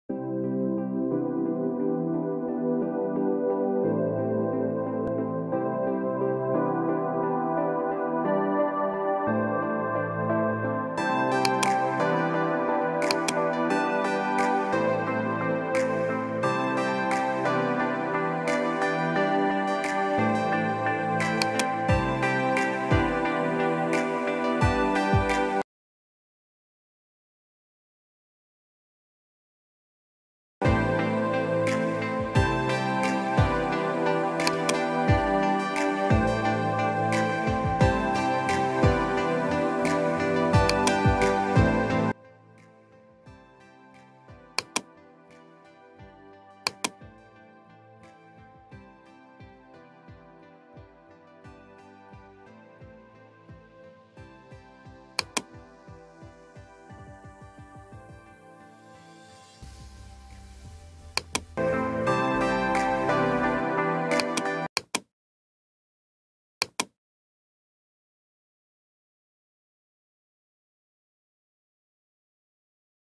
How to Add Audio/Music and Adjust audio/Music Volume in Wondershare Filmora9 | 2020
In this Tutorial you will find How to Add and Adjust volume in Wondershare Filmora